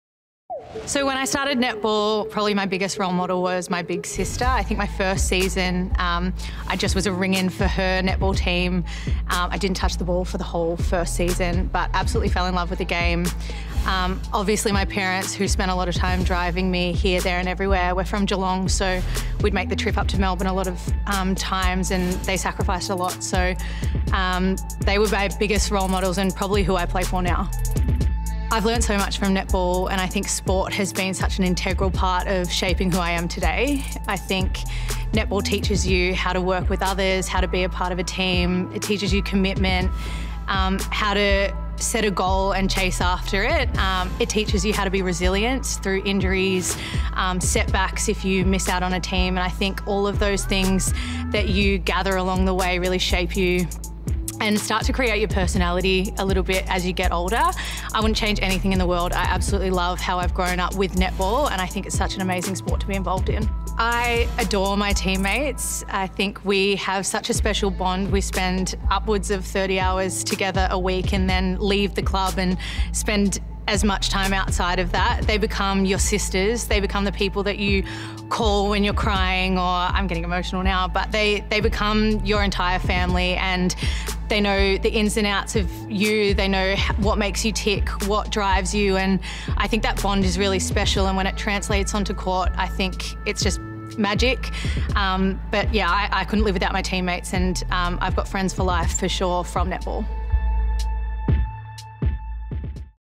In Their Corner | Kelsey Browne
Thanks to Suncorp, the ‘In Their Corner’ series showcases a selection of stars as they reflect on the importance of the people who supported or inspired them, and what they’ve learnt on their netball journey.